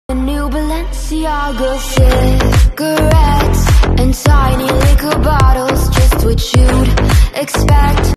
Gaur (Bos gaurus) or Indian bison is the largest species of wild cattle, Natural Wildlife
The gaur (Bos gaurus) or Indian bison is the largest species of wild cattle, larger than the African buffalo, the extinct aurochs and the wild water buffalo, and among the largest living land animals.